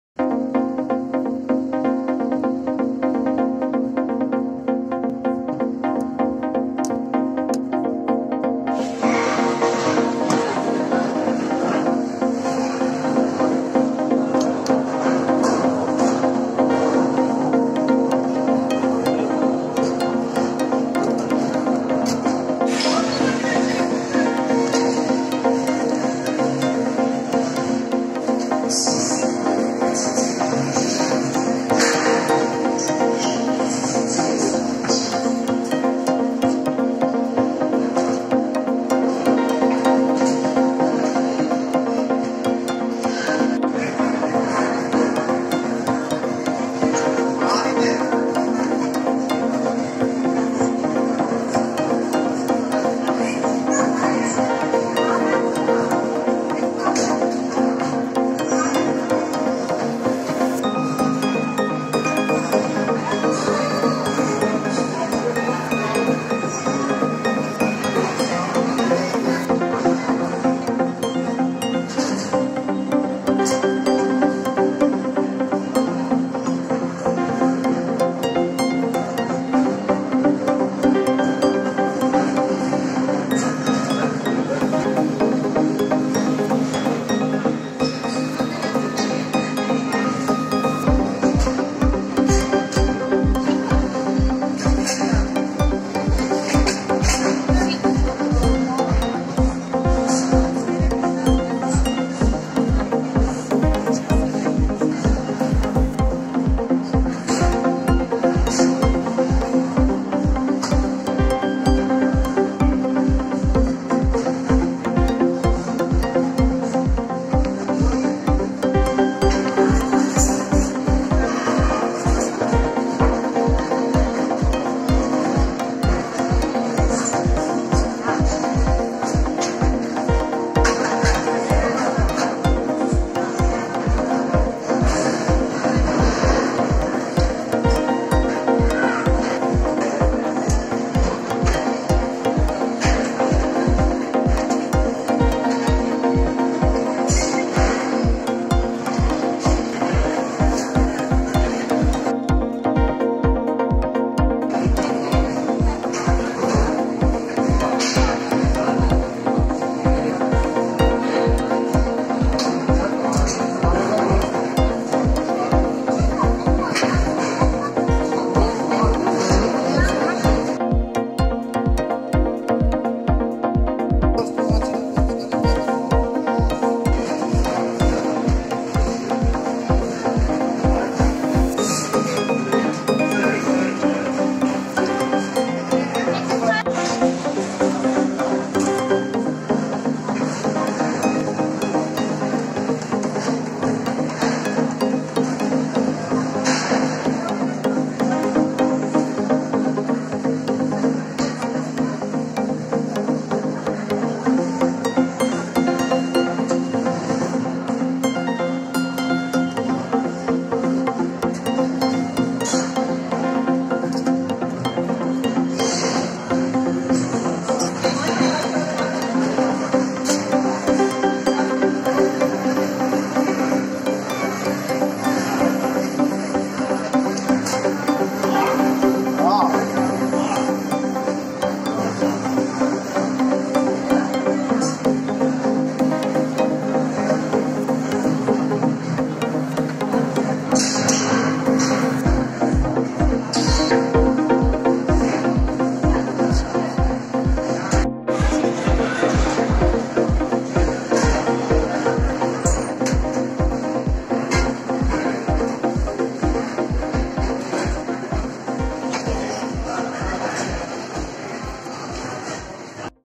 SAS Food Court قاعة طعام ساس
.زيارة الى
في هذا الفيديو ستتعرف وتشاهد انوا مختلفة من المعجنات والاكلات والسلطات الارمنية .
فطائر البيراشكي ، خبز لافاش، انواع السلطات، مختلف المعجنات، عصائر مختلفة
lavash bread